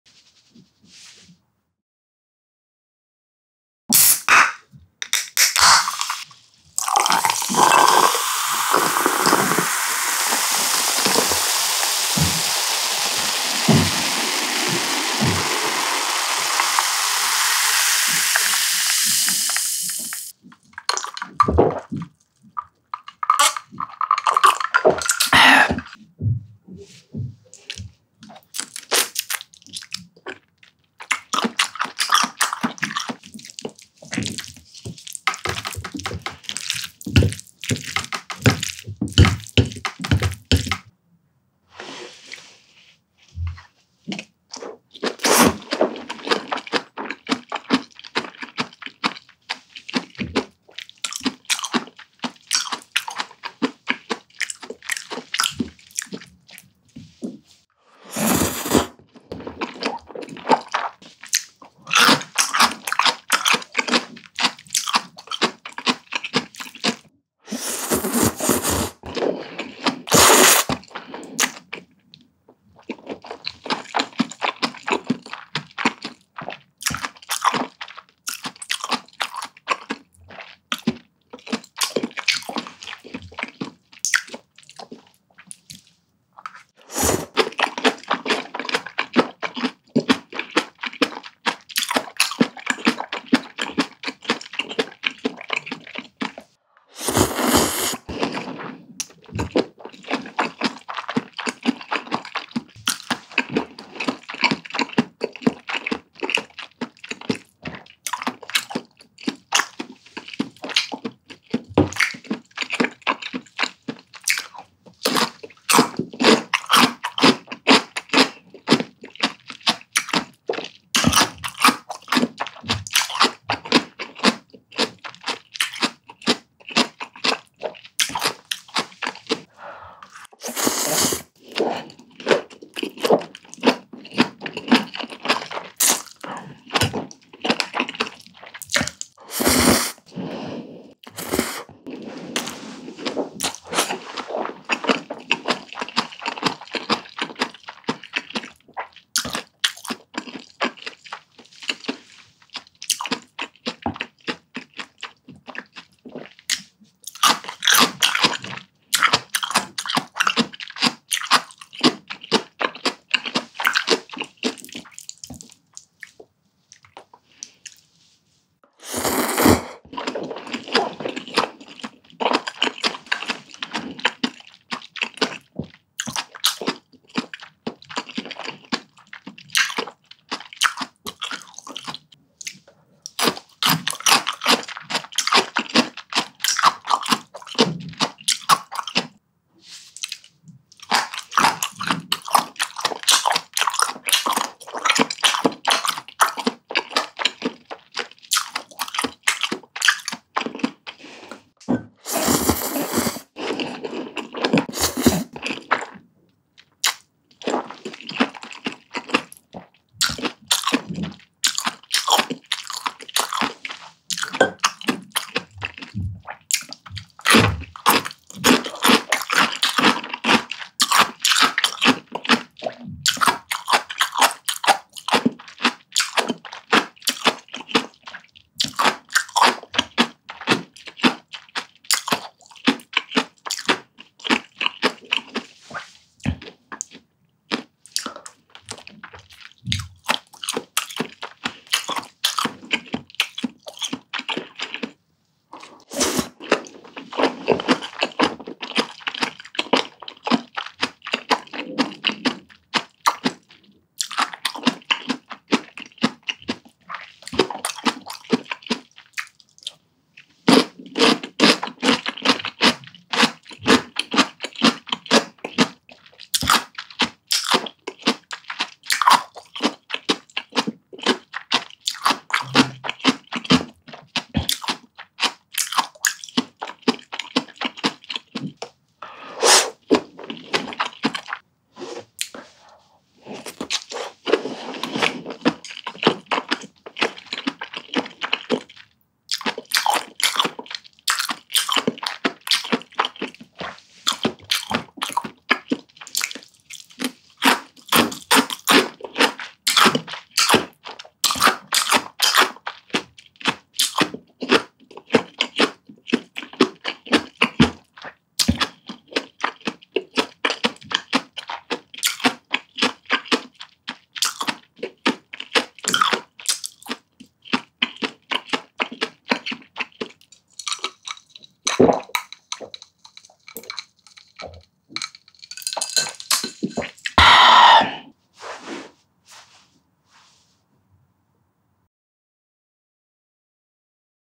팟타이 먹방 ASMR (Pad thai MUKBANG ASMR)
오늘은 팟타이를 준비했어요
Today, I prepared Pad thai.